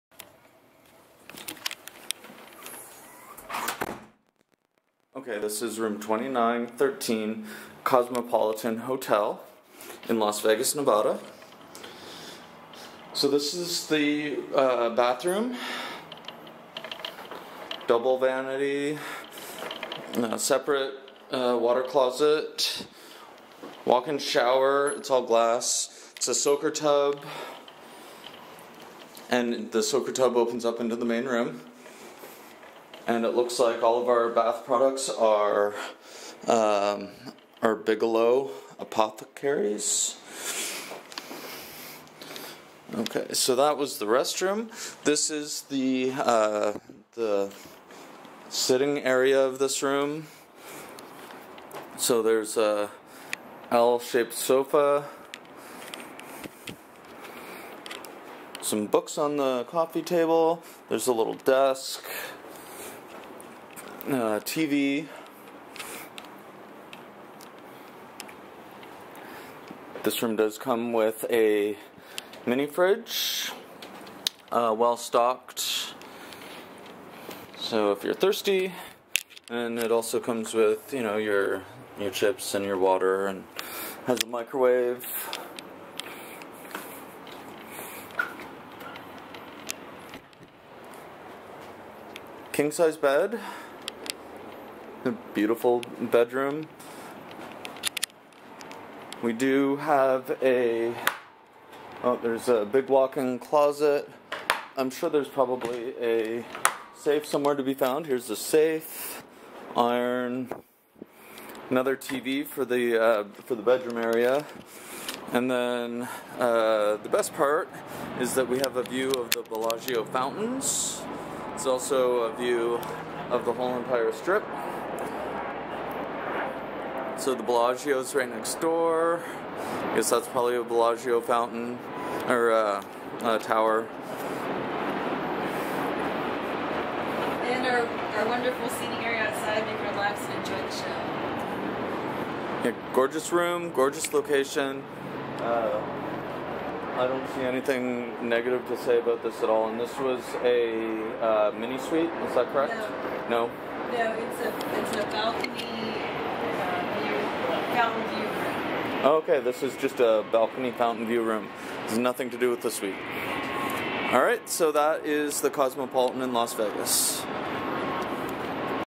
Cosmopolitan of Las Vegas Terrace Studio Fountain View Room w/gorgeous outside strip view
We decided to indulge ourselves by staying at the Cosmopolitan. We picked an outdoor terrace studio room with a spectacular view of the Bellagio fountains and a gorgeous strip view.  It was amazing during the day and trul Cannot beat this view at night!  We lucked out and got a great deal on a Thursday night so although it was a treat it did not cost the moon like it will on high season nights.